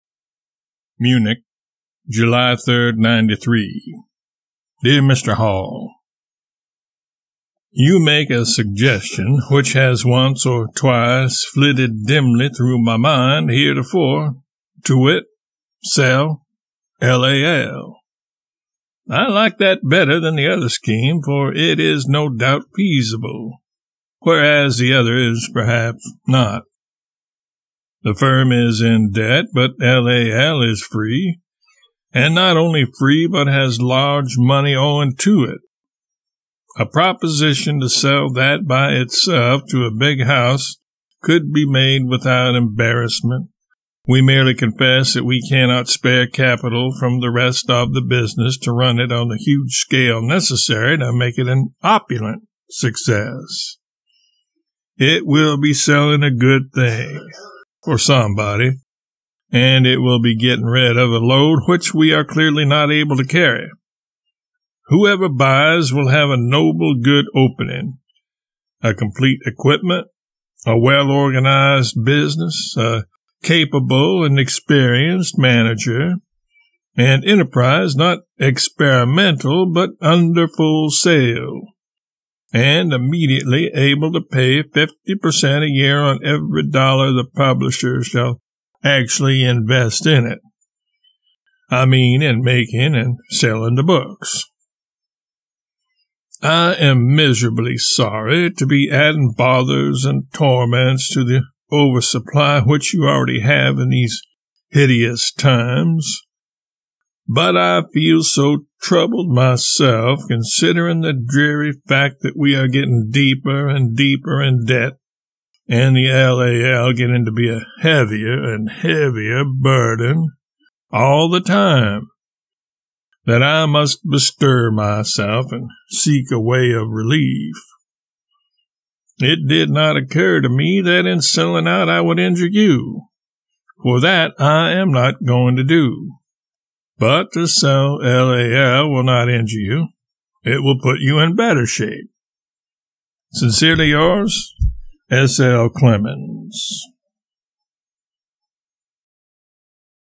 July 3, 1893 Letter from Mark Twain to Fred J. Hall_271
July 3, 1893 Letter from Mark Twain to Fred J. Hall, Realized by Mark Twain interpreter B. Clay Shannon